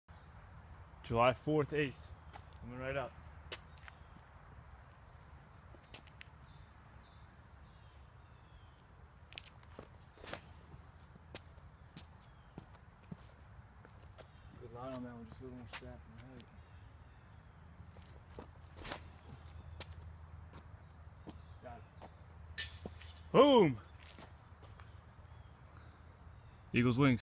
July 4th Ace with the Star Valkyrie
Hole 10 at Buchmiller Park in Lancaster PA